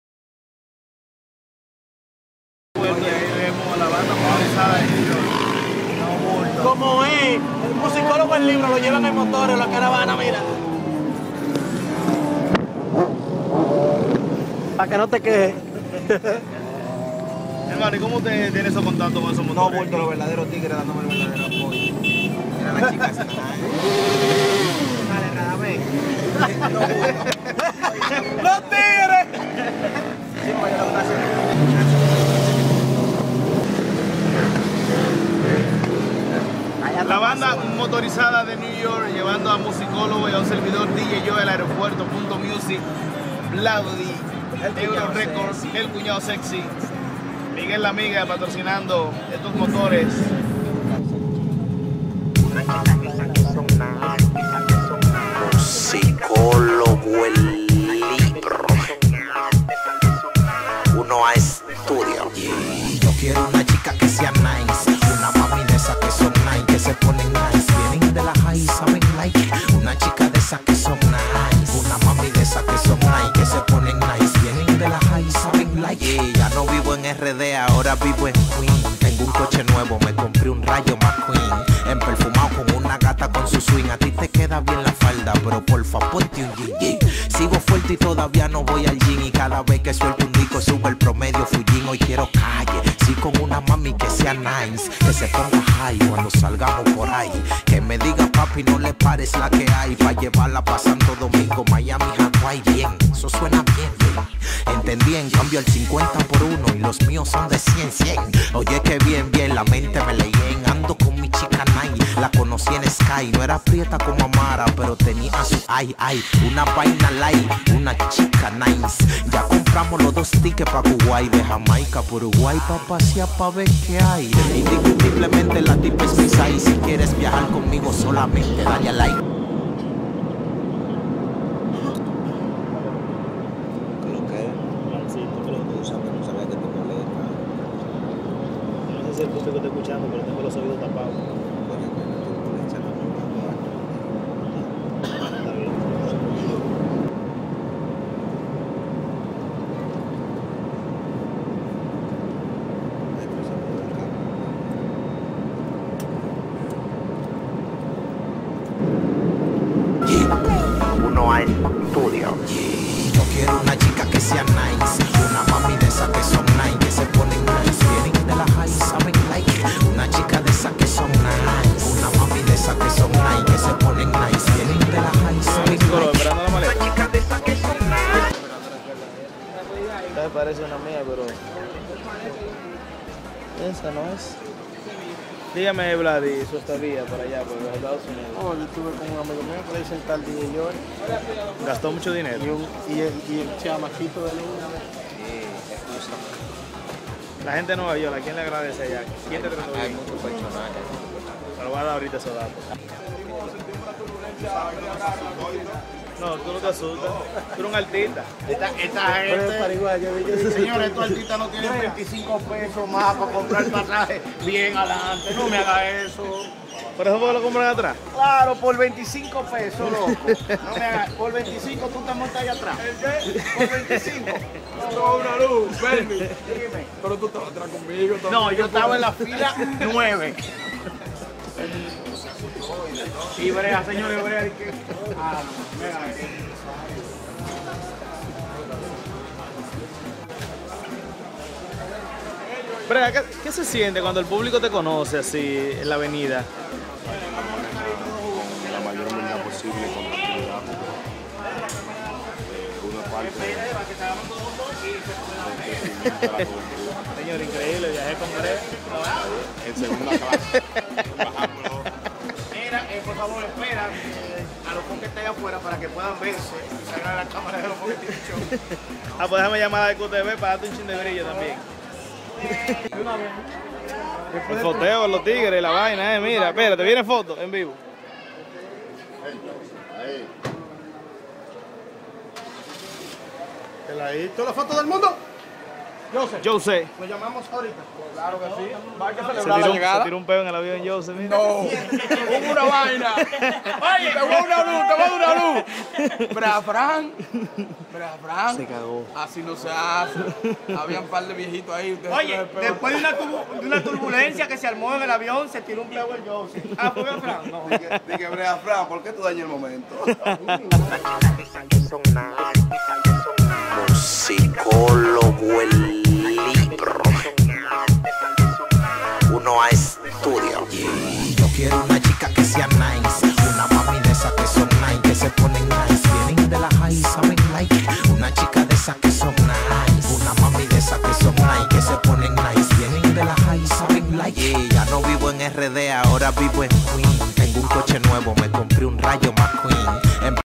Musicologo y Dj Joe en su Salida de Ny Llegada a Rd junto a Brea Frank, Melvin de leon, el Jou-c
En su salida de ny fueron acompañados por una banda motorizada presentan el avion en su salida viajan en el mismo vuelo brea frank, el jouc, melvin el leon, punto music, euro record, uvikate.net, combo de los 15